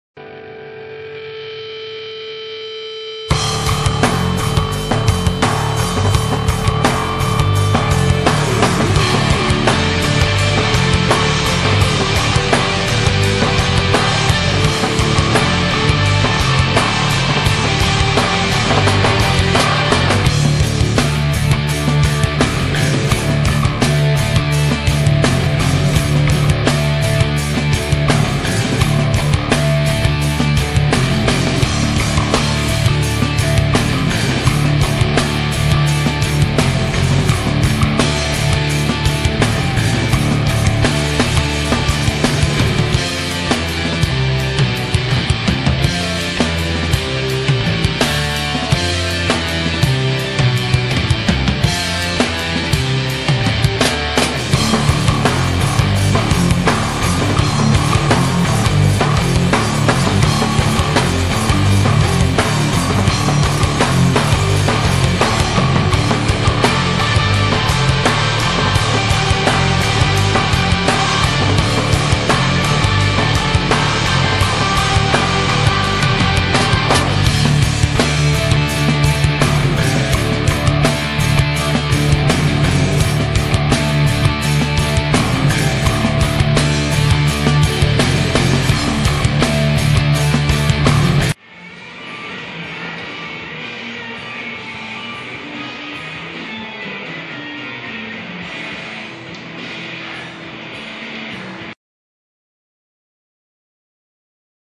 Intense Fat Burning,Lean Muscle MMA Endurance Workout: Macebell Shredder MMA Circuit Preview
The XFunction Macebell Shredder MMA circuit is designed to burn fat and build lean muscle for elite athletes and fitness enthusiasts.  Use a simple Macebell and your body to train anywhere you can swing and move!  The entire Macebell Shredder program is forthcoming on DVD and via digital download